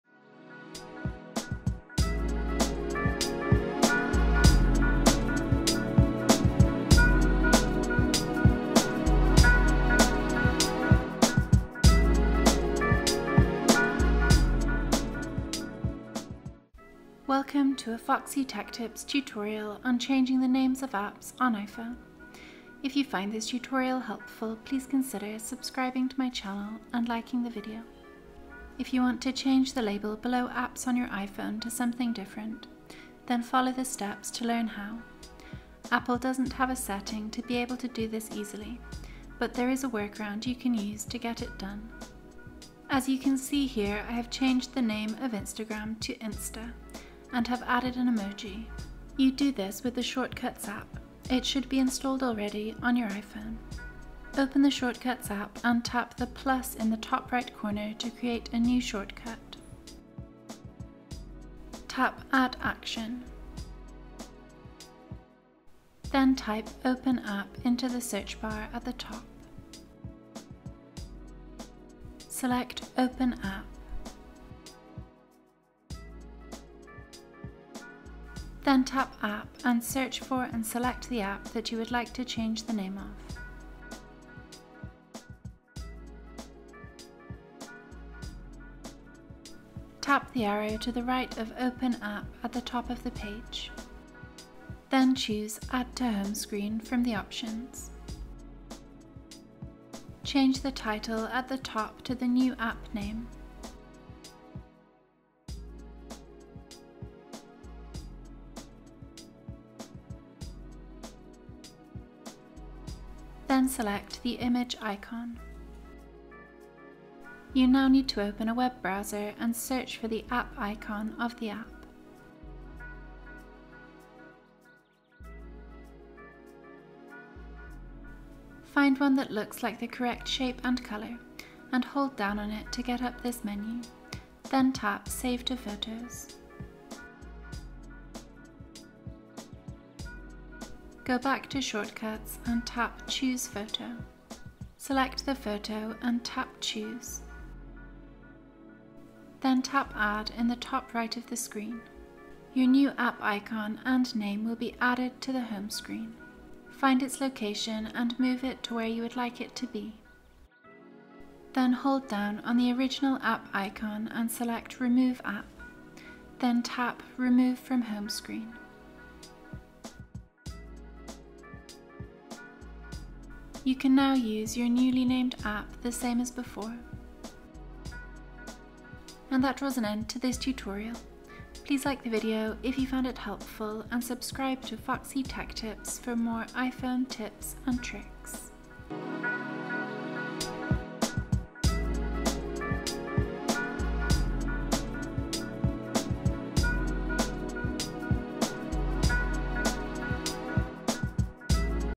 iPhone: Change App Names
Easy to follow tutorial on changing the names of apps on iPhone. If you want to change the label below apps on your iPhone to something different then follow the steps to learn how. Apple doesn't have a setting to be able to do this easily, but there is a workaround you can use to get it done.

Steps:

Open the shortcuts app and tap the plus in the top right corner to create a new shortcut.

Tap ‘add action’.

Then type ‘open app’ into the search bar at the top.

Select ‘open app’.

Then tap ‘app’ and search for and select the app that you would like to change the name of.

Tap the arrow to the right of ‘open app’ at the top of the page.

Then choose ‘add to home screen’ from the options.

Change the title at the top to the new app name.

Then select the image icon.

You now need to open a web browser and search for the app icon of the app.

Find one that looks like the correct shape and color and hold down on it to get up this menu, then tap ‘save to photos’.

Go back to shortcuts and tap ‘choose photo’.

Select the photo and tap ‘choose’.

Then tap ‘add’ in the top right of the screen.

Your new app icon and name will be added to the home screen.

Find its location and move it to where you would like it to be.

Then hold down on the original app icon and select ‘remove app’ then tap ‘remove from home screen’.